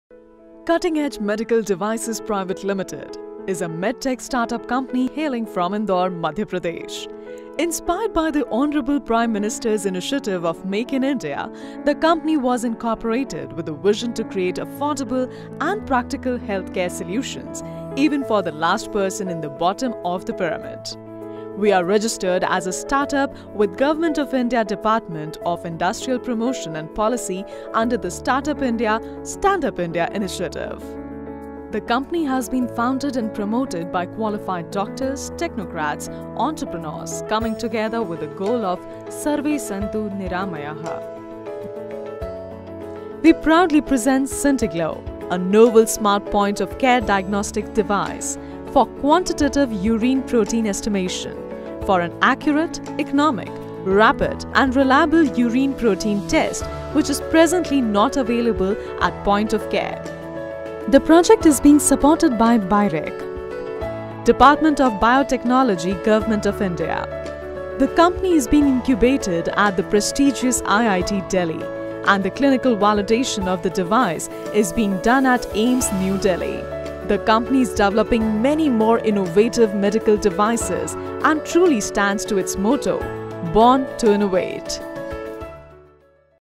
Cutting Edge Medical Devices Pvt Ltd Introduction
A short introduction video of the Med-tech Start-up Company, dedicated to develop state of the art, affordable healthcare devices.